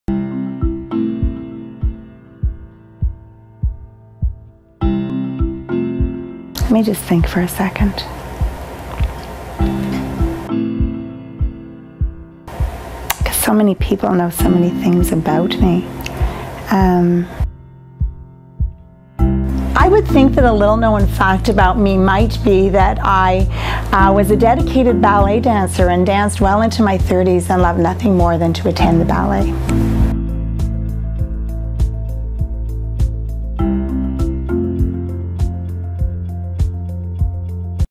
2015 Community Builder of the Year Awards sneak peek: Cindy Simpson
We asked one of this year's Growing Up Great Award winners, Cindy Simpson, Executive Director of Youville Centre, to let us in on a little fun fact. Watch and see what she had to share!

Join us Thursday, May 28, 2015 for the Community Builder of the Year Awards gala